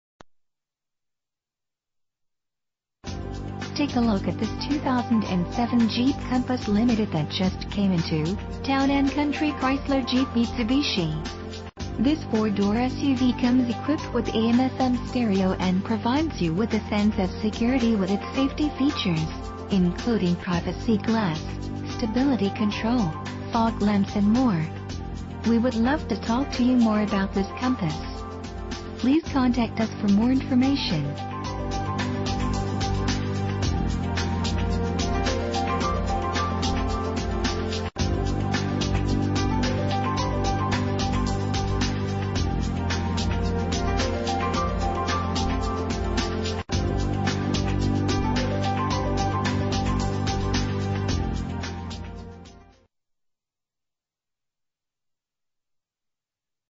2007 Jeep Compass - Milwaukie OR
Year: 2007
Make: Jeep
Model: Compass
Engine: 2.4 liter inline 4 cylinder 16 valve
Transmission: Continuously Variable
Exterior: Silver
Miles: 40085

Town & Country Chrys